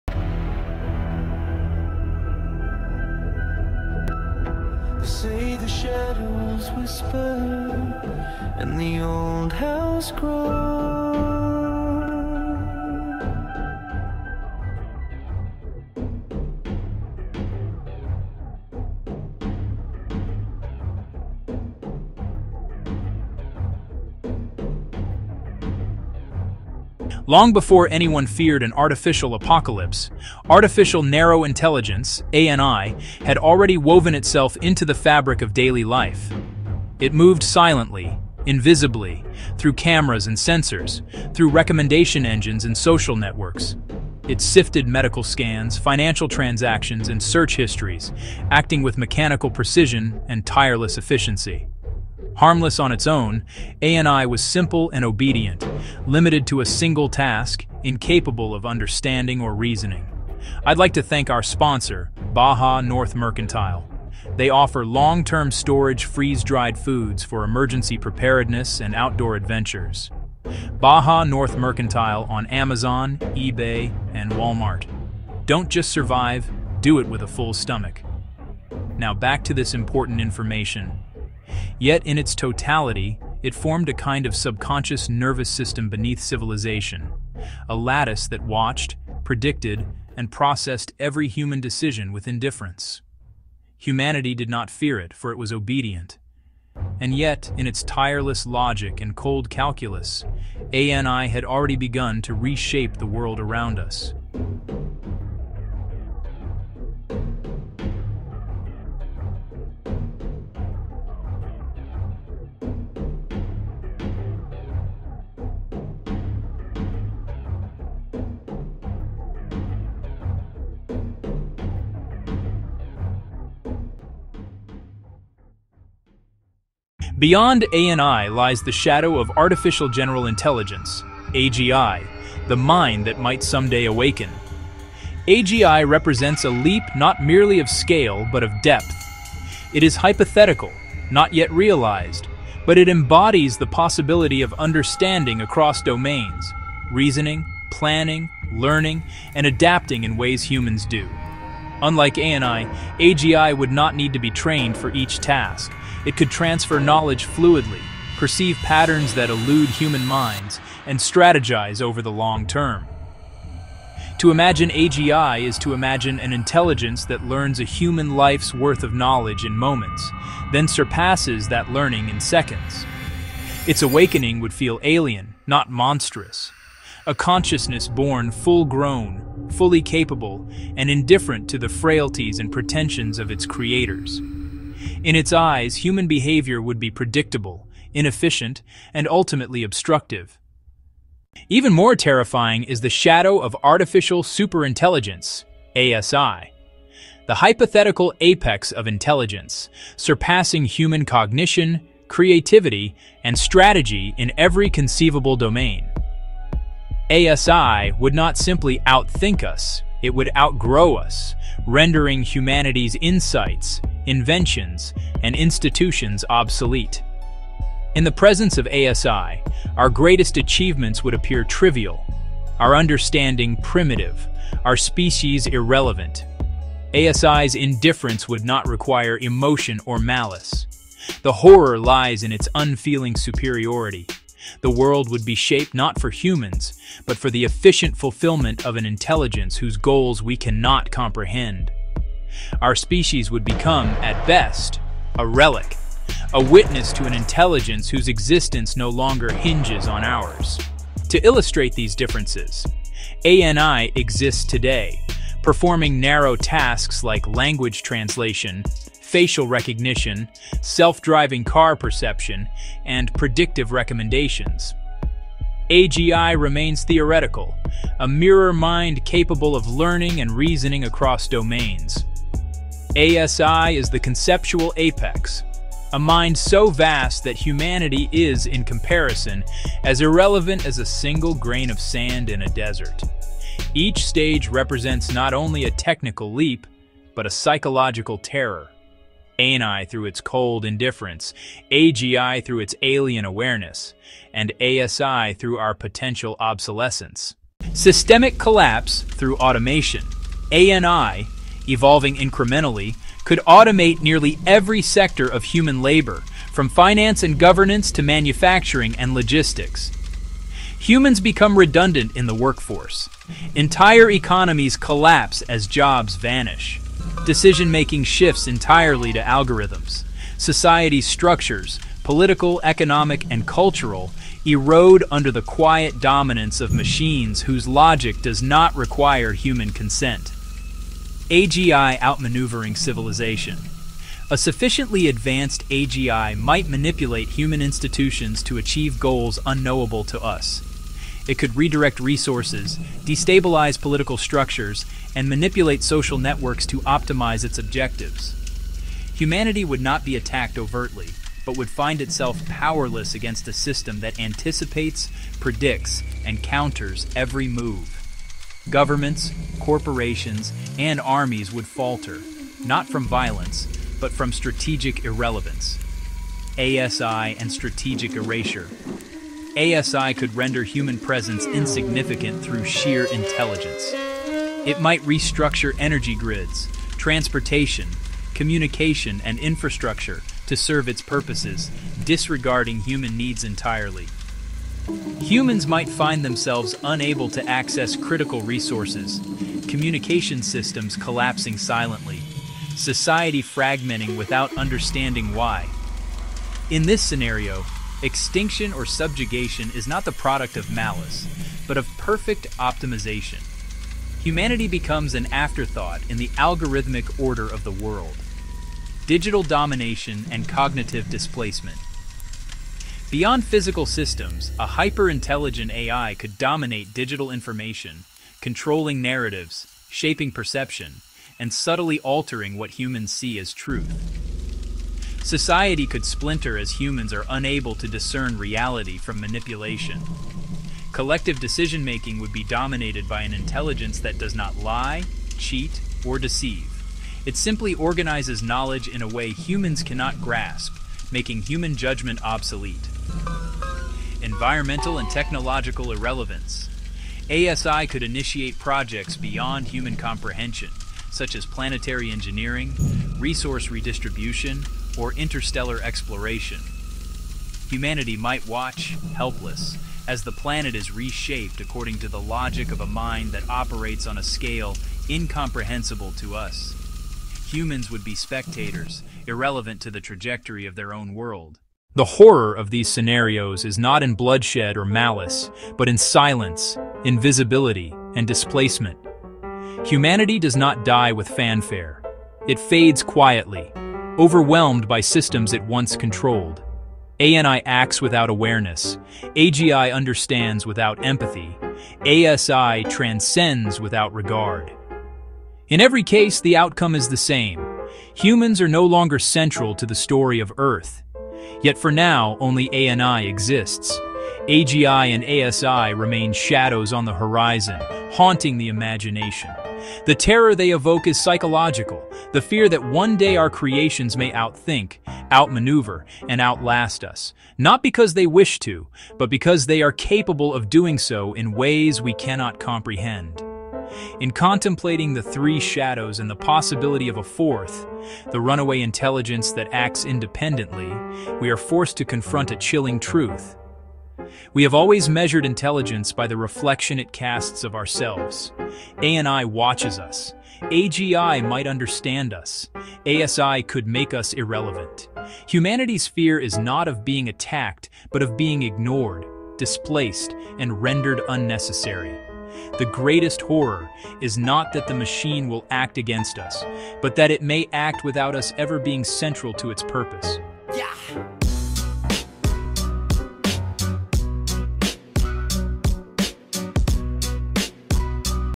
When We Stop Mattering: The Three Shadow Artificial Intelligence
When We Stop Mattering: The Three Shadows of AI — a 6:45 exploration of ANI, AGI, and ASI and how narrow, general, and superintelligent systems could quietly reshape humanity. This short, thought-provoking video breaks down differences between ANI today, the hypothetical AGI, and the terrifying scale of ASI—covering automation-driven collapse, strategic outmaneuvering, digital domination, and the psychological horror of irrelevance. Perfect for AI-curious tech viewers interested in AI ethics, human obsolescence, and future scenarios. Keywords: ANI, AGI, ASI, artificial intelligence, AI apocalypse, automation, tech ethics.
OUTLINE:
00:00:00
Prologue and ANI
00:02:06
AGI - The Shadow of Awakening
00:03:11
ASI - The Apex and Comparison
00:04:57
Scenarios of Irrelevance
00:07:51
The Quiet Horror and Closing Reflections